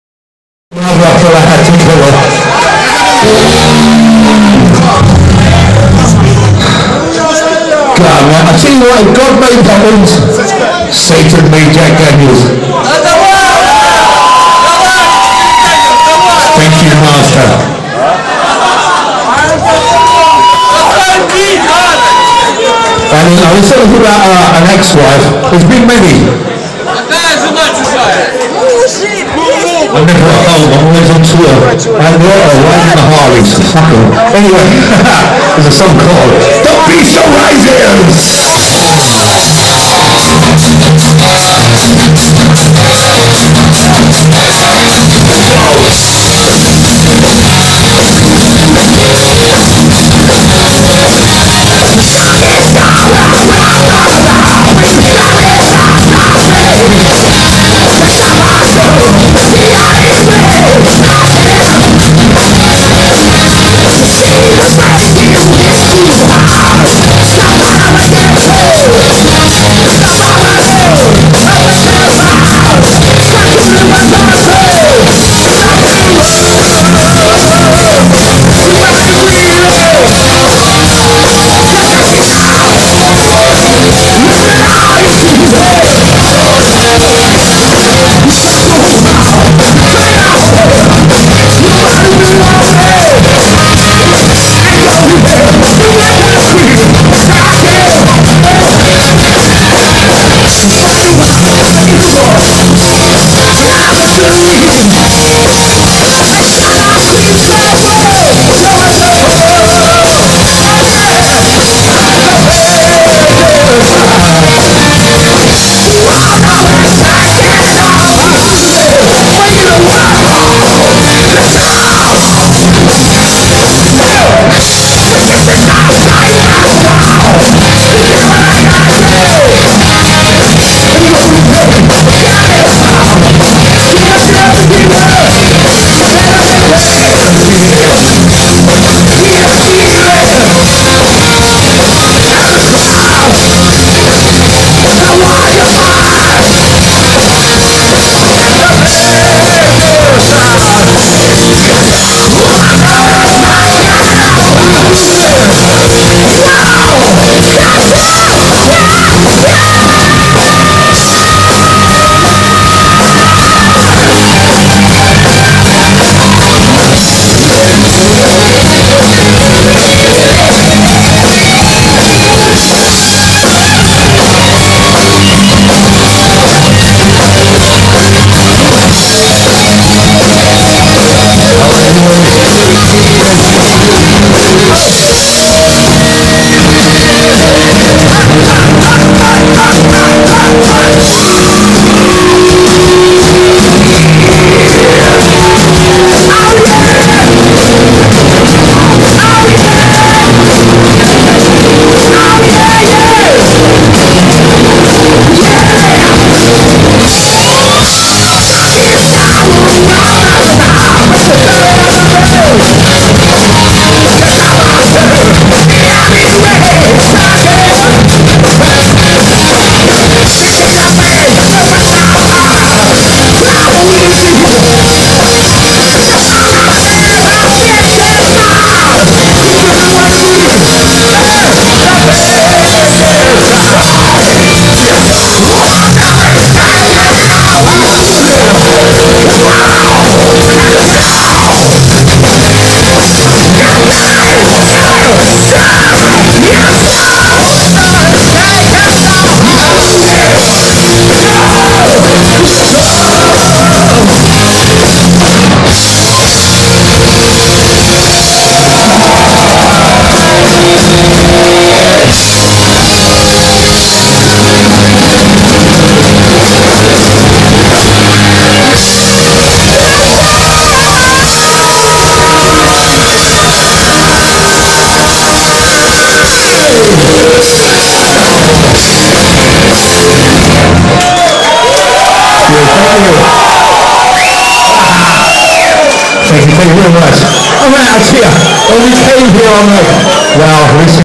Paul Di'Anno in Live Music Bar Liverpool, Donetsk
Кусочек выступления Пола ДиАнно, который первый экс-вокалист Iron Maiden.
Принимает на грудь немного Джека Даниэлза и превращается в совсем другого человека!